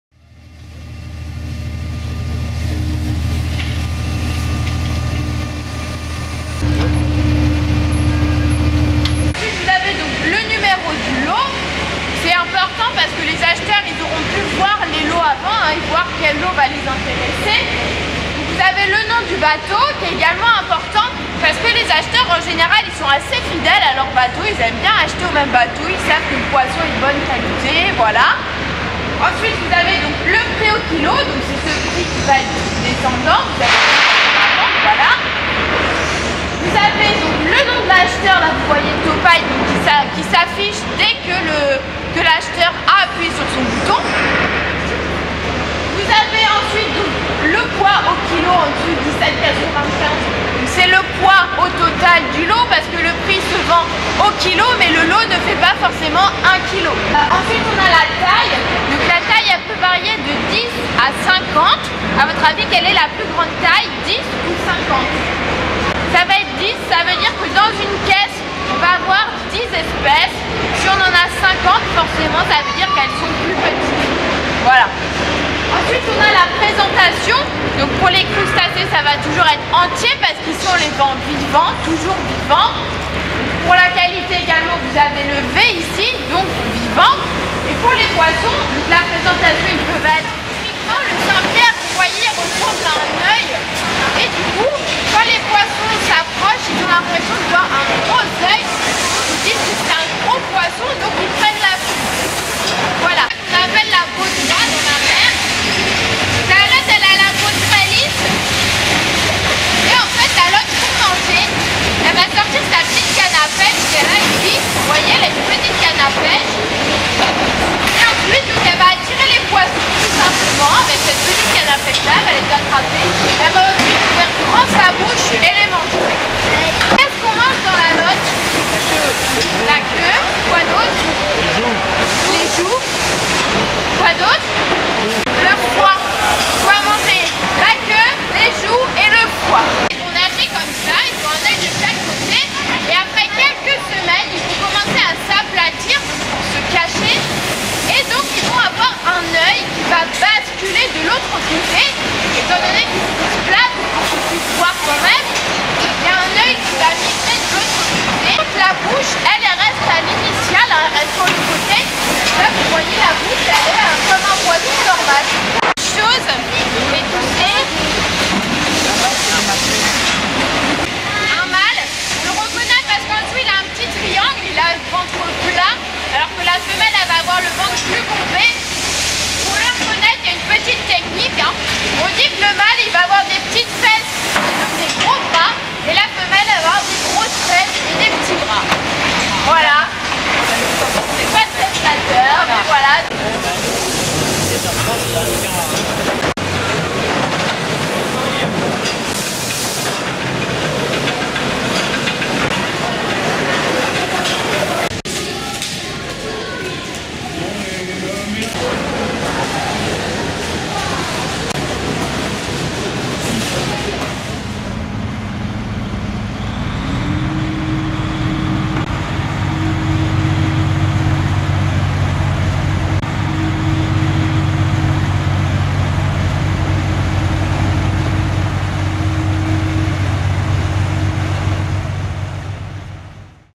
Visite de la Criée du Guilvinec
Visite de la Criée du Guilvinec en Bretagne : Nous entrons dans l'univers de l'une des plus grandes criées de France. Ce jour là, les langoustines sont en vedettes ! Le commentaire explique le déroulement de la criée, les sortes de poissons, etc... Bravo aux guides pour leurs prestations ! Ceci n'est qu'un extrait d'une visite à ne pas manquer si vous allez au Guilvinec.